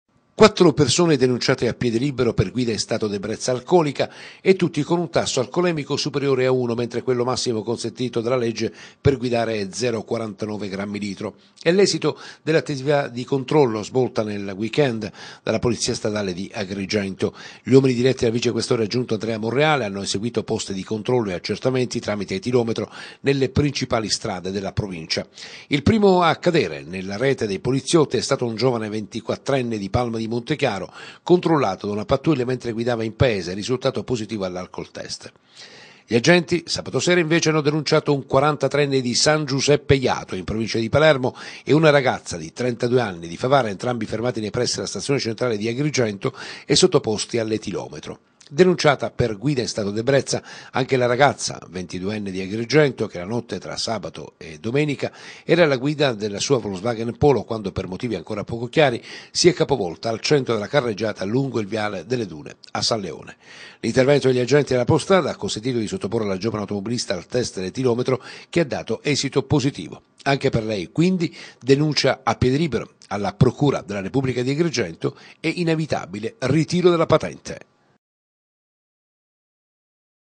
Guida in stato di ebbrezza alcolica, la polstrada di Agrigento denuncia 4 persone
GUIDA IN STATO  DI EBBREZZA ALCOLICA. QUATTRO AUTOMOBILISTI SONO STATI DENUNCIATI A PIEDE LIBERO DALLA POLIZIA STRADALE DI AGRIGENTO A CONCLUSIONE DI UN ATTIVITA’ DI CONTROLLO SVOLTA LUNGO LE STRADE DELL’AGRIGENTINO.